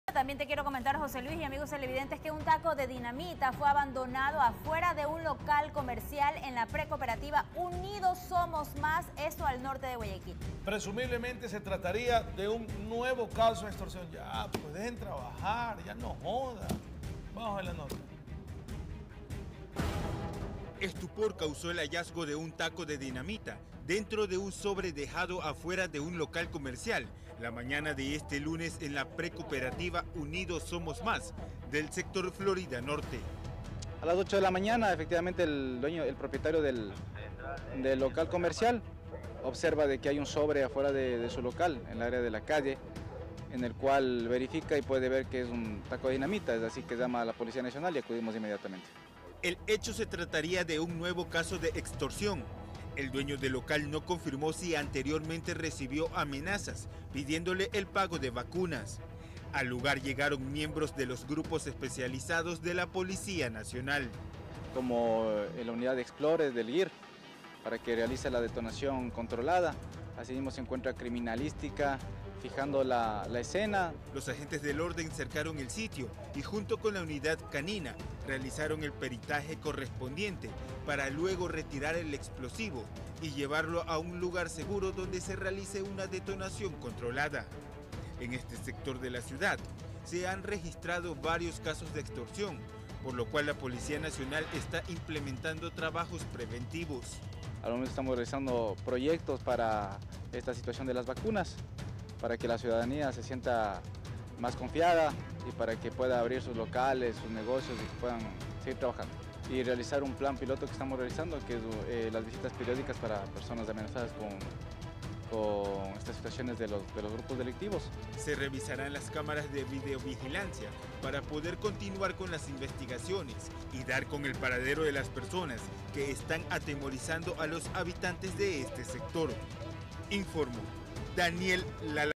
Dejan taco de dinamita en local comercial al norte de Guayaquil
El hallazgo causó preocupación en los vecinos del lugar que esperaron a que las autoridades competentes se hicieran cargo del artefacto. En esta zona, se han registrado varios casos de extorsión, por lo que la Policía se encuentra haciendo trabajos preventivos.